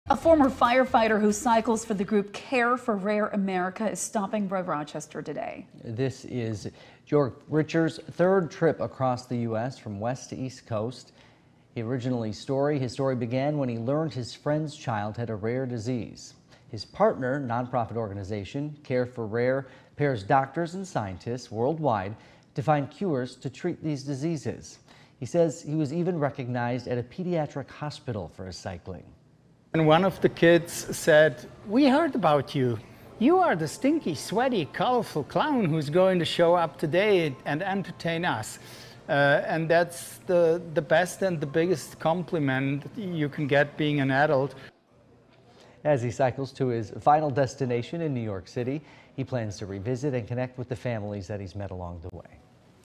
Cyclist on cross-country trip to raise awareness
(ABC 6 News) - A former firefighter who cycles for the group 'Care for Rare America' stopped in Rochester Monday.

Jorg Richter is currently on his third trip cycling across the United States from west to east coast.

Richter's story began when he learned his friend's child had a rare disease. The non-profit organization pairs together doctors and scientists to help cure children's rare diseases.

Richter says he was even recognized at a pediatric hospital for his cycling.

"When one of the kids said we heard about you, you are the stinky, sweaty, colorful clown who's going to show up today and entertain us. And that's the best and biggest compliment you can get being an adult." Richter said.

This trip includes Richter passing by the many cities across the United States along with stops in Minneapolis and our southeastern region.

As Richter cycles to his final destination in New York City, he plans to revisit and connect with the families he has met along the way.